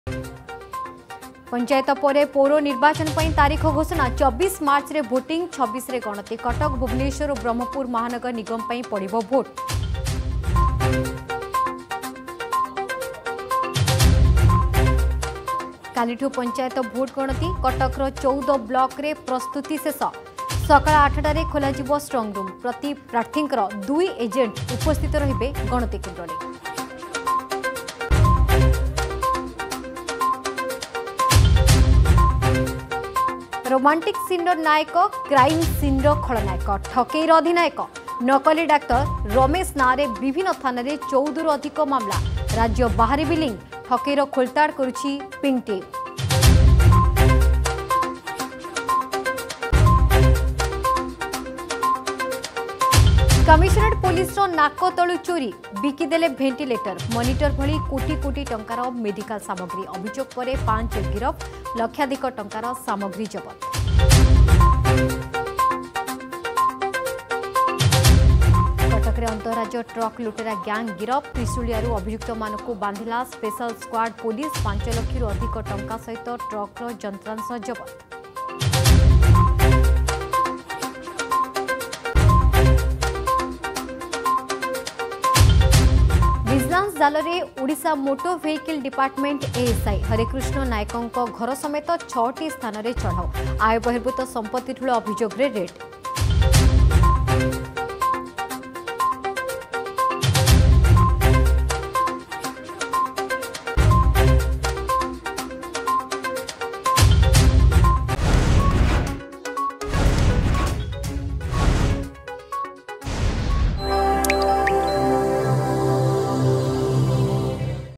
Headlines 6 | 25 Feb 2022 | Argus News
+ପଞ୍ଚାୟତ ପରେ ପୌର ନିର୍ବାଚନ ପାଇଁ ତାରିଖ ଘୋଷଣା
+ କାଲିଠୁ ପଞ୍ଚାୟତ ଭୋଟ୍ ଗଣତି
+ରୋମାଣ୍ଟିକ ସିନର ନାୟକ, କ୍ରାଇମ ସିନର ଖଳନାୟକ, ଠକେଇର ଅଧିନାୟକ
+ କମିସନରେଟ୍ ପୋଲିସର ନାକତଳୁ ଚୋରି
+ କଟକରେ ଅନ୍ତରାଜ୍ୟ ଟ୍ରକ ଲୁଟେରା ଗ୍ୟାଙ୍ଗ ଗିରଫ
+ ଭିଜିଲାନ୍ସ ଜାଲରେ ଓଡ଼ିଶା ମୋଟର ଭେଇକିଲ ଡିପାର୍ଟମେଣ୍ଟ ଏଏସଆଇ